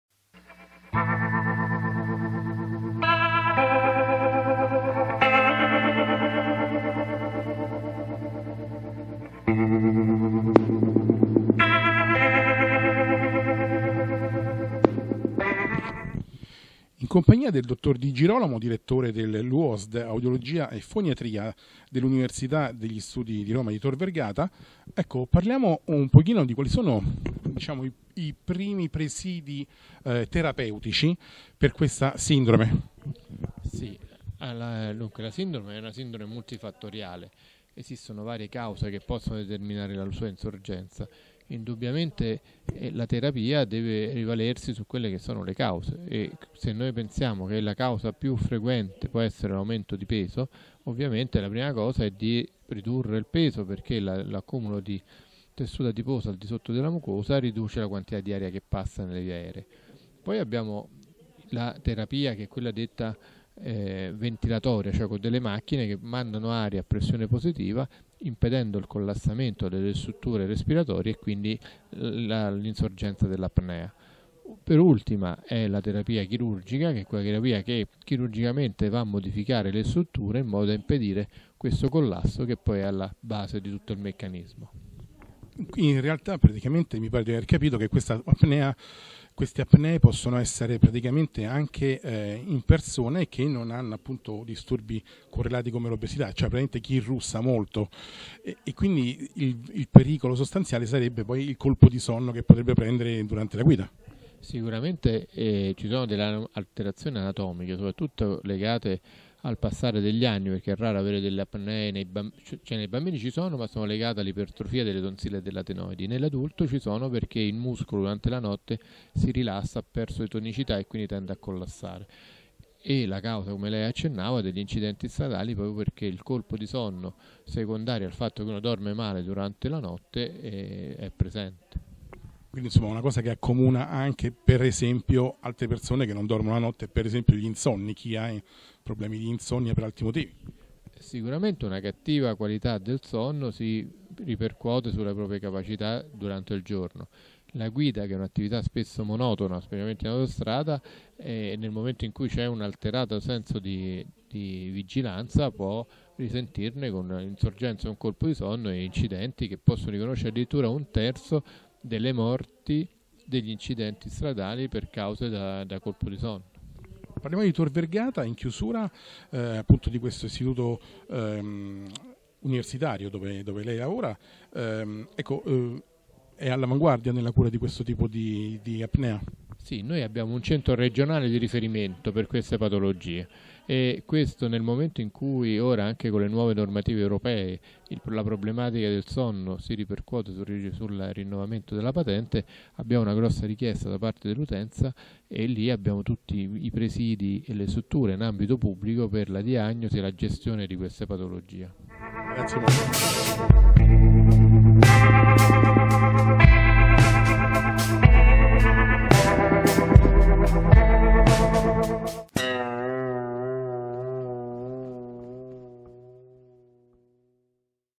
Stefano Di Girolamo - Seminario "OSAS Salute e Sicurezza stradale"
Registrazione effettuata da liberi.tv il 9/11/2016 presso la Sala Zuccari del Senato.

Seminario "OSAS - Salute e Sicurezza stradale" - II Giornata 2016
organizzato dall'associazione L'Alba del Terzo Millennio

Intervista a Stefano Di Girolamo, Direttore della UOSD Audiologia e foniatria dell'Università degli Studi "Tor Vergata".